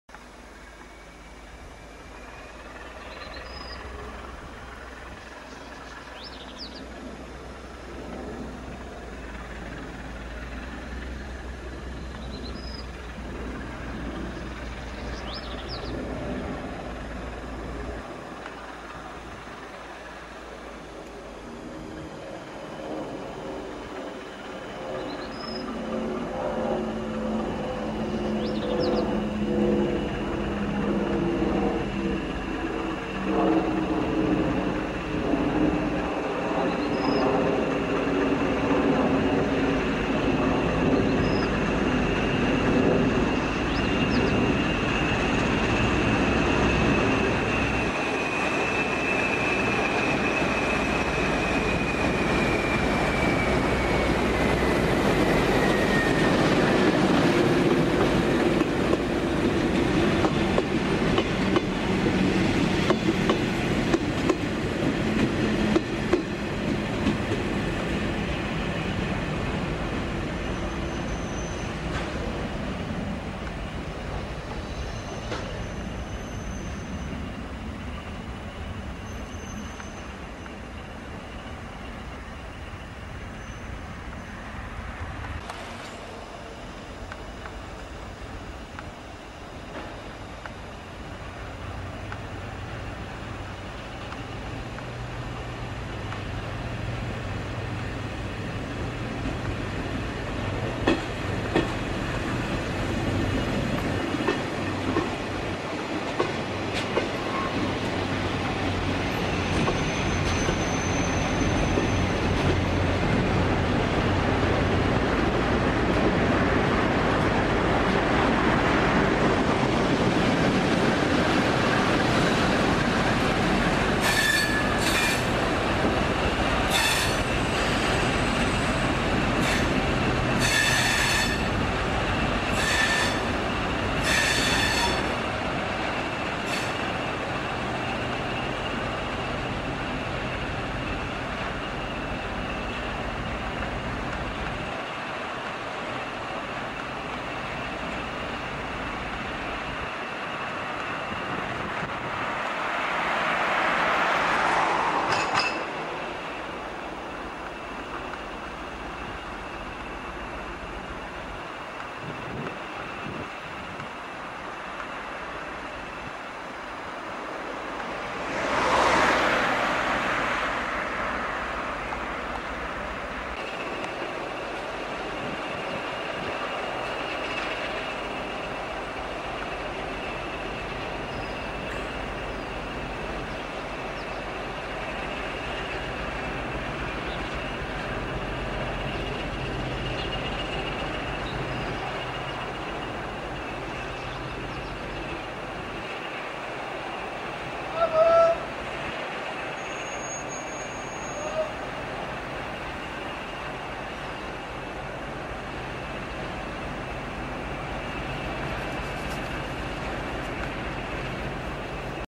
CP 1413 + UTE 3153 -» Entrada na Oficina de Oeiras -» 21 Mar 2013
Mais uma andança desta 1400 com o material da Linha de Cascais, proveniente de Campolide-A, desta vez para Oeiras, com a UTE 3153 com alguns boggies a brilhar.